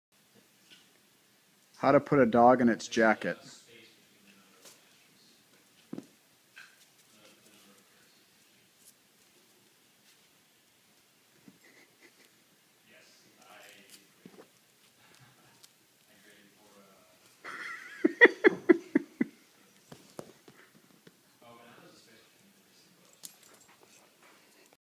Putting a poodle's jacket on
A creative way my son found to put a poodle's jacket on